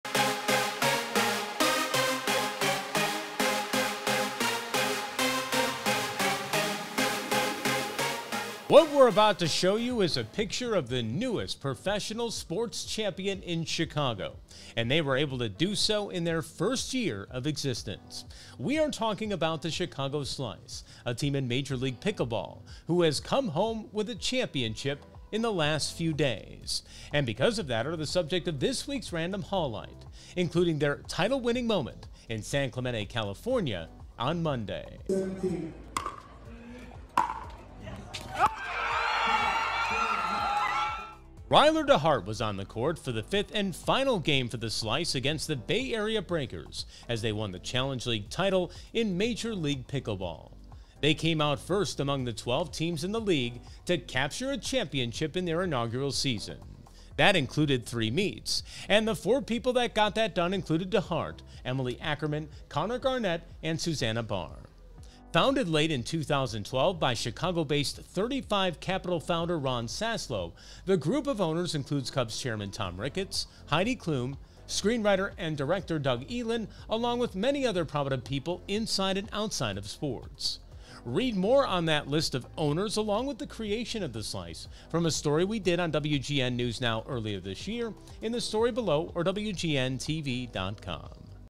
See Chicago's newest pro sports champion on the "Random Hawlight"
Read more on the championship for the Chicago Slice of Major League Pickleball